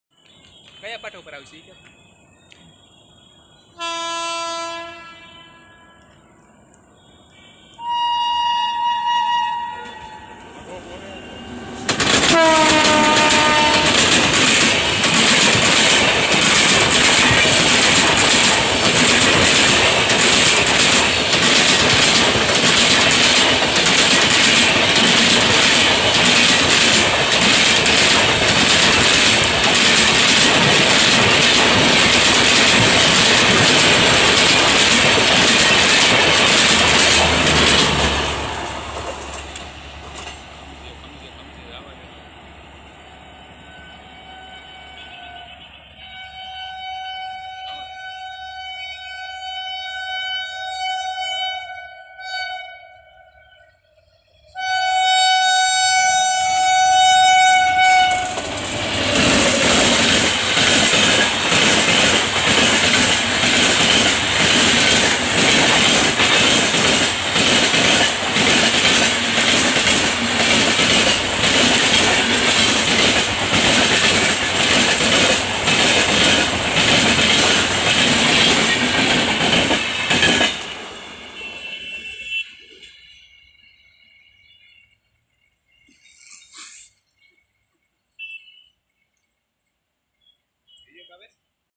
2 TRAIN IN 1 || KARNAVATI EXPRESS CROSSED SHATABDI EXPRESS || TRAIN VIDEO
Karnavati Express -12933 ( Mumbai Central to Ahmedabad Junction )
Classes: CC,2S
Service Days: Monday, Tuesday, Wednesday, Thursday, Friday, Saturday, Sunday
Stops:11
 Duration:7 hrs 45 mins Type:Mail/Express
Pantry:yes

Shatabdi Express -12010 ( Ahmedabad Junction to Mumbai Central )
Classes: 1A,CC
Service Days: Monday, Tuesday, Wednesday, Thursday, Friday, Saturday
Stops:9 Duration:7 hrs 5 mins Type:Shatabadi Pantry:yes